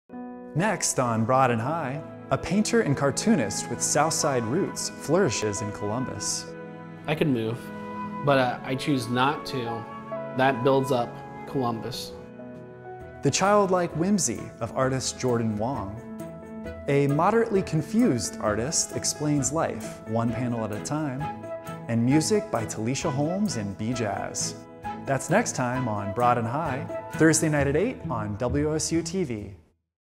Cartoonists, Painters And Illustrators Preview
Bryan Moss, a painter and cartoonist with South Side roots, flourishes in Columbus. The childlike whimsy of artist Jordan Wong. Jeff Stahler, creator of the “Moderately Confused” cartoon, explains life, one panel at a time, and music from Talisha Holmes & BJazz.